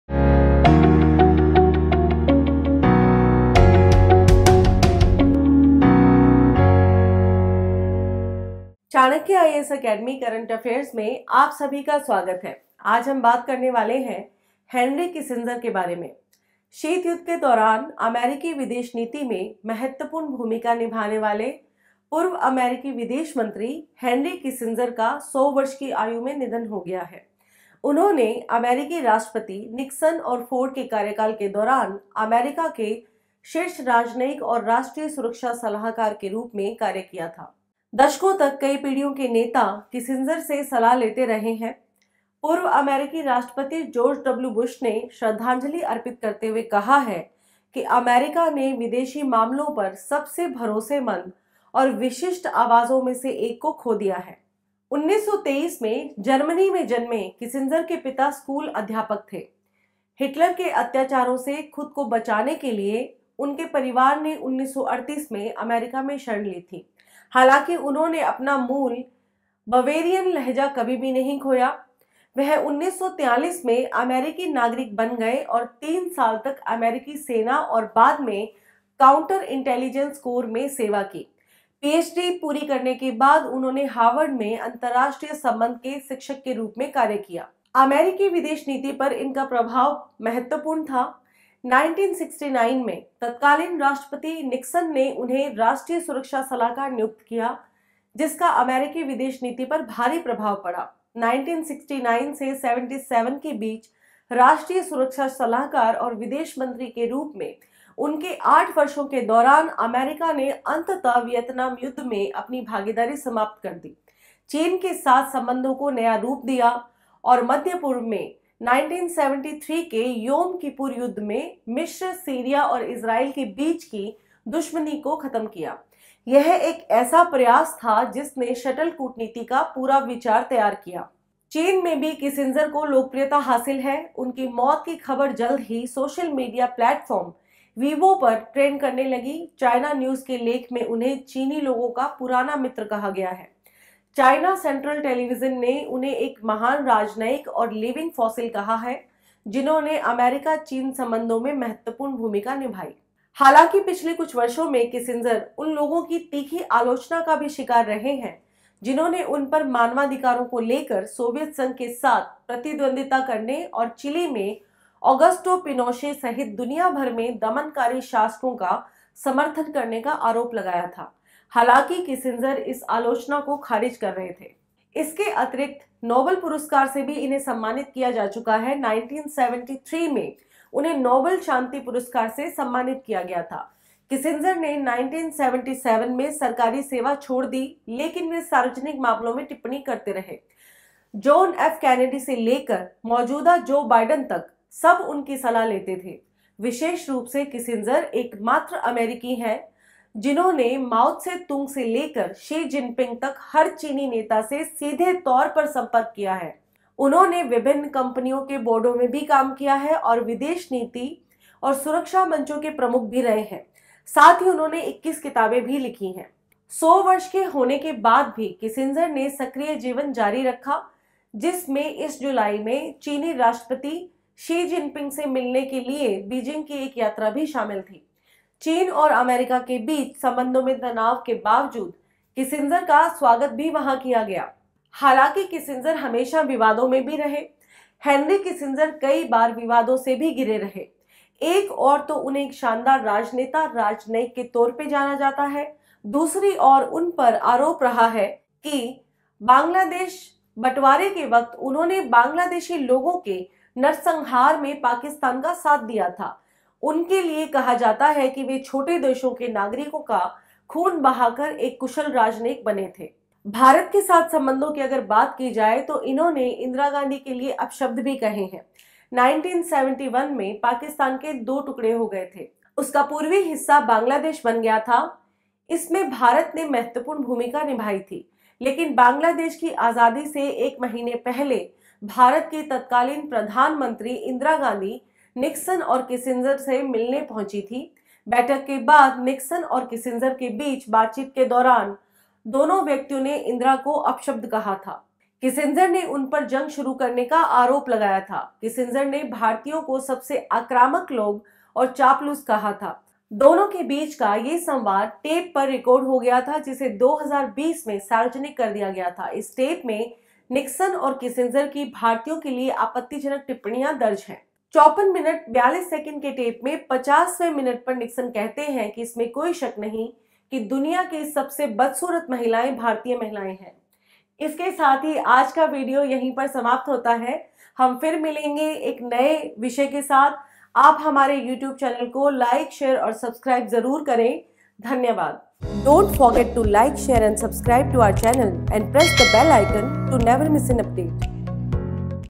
हेनरी किसिंजर: भारत का दुश्मन, चीन का दोस्त । Chanakya' Current Affairs
किसिंजर 1973 से 1977 तक अमेरिका के 2 पूर्व राष्ट्रपति रिचर्ड निक्सन और जेराल्ड फोर्ड के कार्यकाल में विदेश मंत्री थे। हालांकि उस दौर में उनका भारत के साथ कई मुद्दों पर विवाद था। 1971 में भारत-पाकिस्तान युद्ध के दौरान किसिंजर ने पाकिस्तान का साथ दिया था। रूसी मीडिया स्पुतनिक के मुताबिक जंग के दौरान राष्ट्रपति निक्सन के कहने पर किसिंजर ने चीन से अपनी सेना को भारत की सीमा के पास तैनात करने को कहा था। अमेरिकी राष्ट्रपति का मानना था कि भारतीय सीमा के पास चीनी सैनिकों की तैनाती से भारत सरकार डर जाएगी और ईस्ट पाकिस्तान (बांग्लादेश) से पीछे हट जाएगी। हालांकि चीन ने ऐसा करने से इनकार कर दिया था। जुलाई 2005 में अमेरिका के विदेश मंत्रालय ने इस जंग से पहले हुई निक्सन और किसिंजर के बीच बातचीत से जुड़े कुछ टेप रिलीज किए थे। न्यूयॉर्क टाइम्स को भी ये टेप मिले थे।